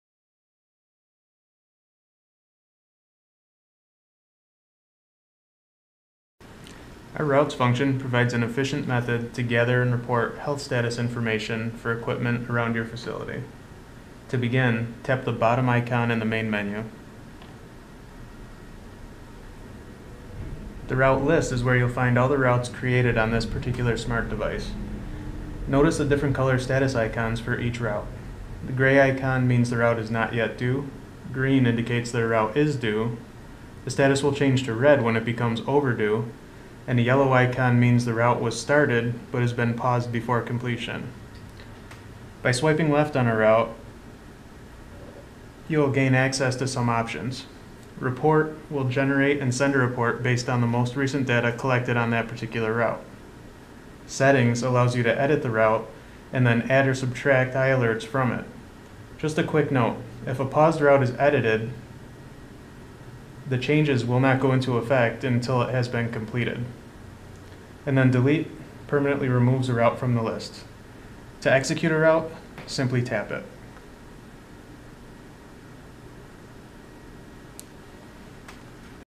Ứng dụng i-ALERT2. Tổng quan về định tuyến
Tổng quan về tính năng định tuyến trong ứng dụng i-ALERT2.